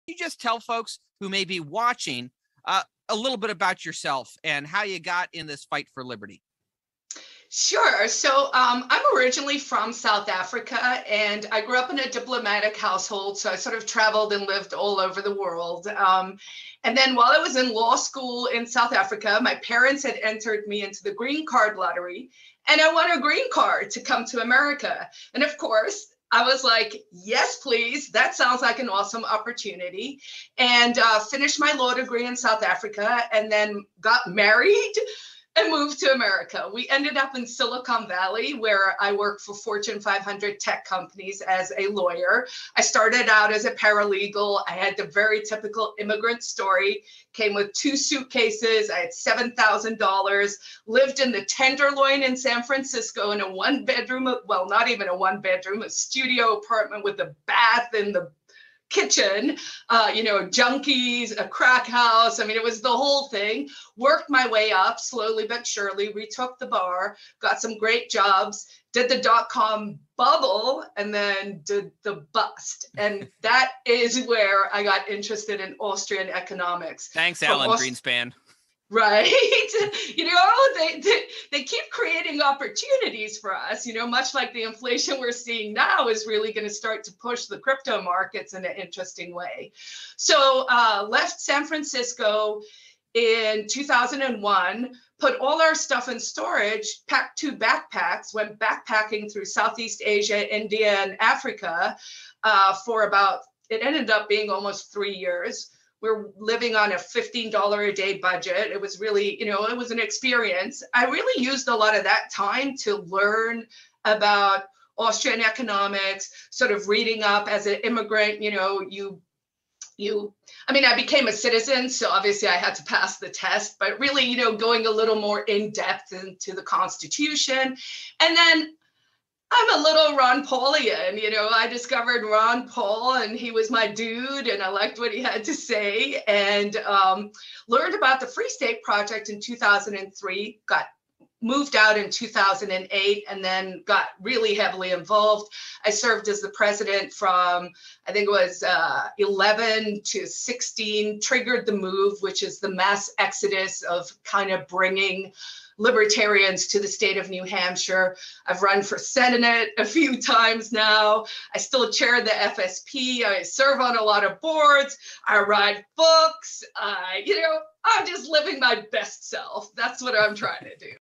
Carla Gericke's Incredible Background in Under 3 minutes!
Carla tells former Maine Senator Erik Brakey about her  life journey to the Free State of New Hampshire.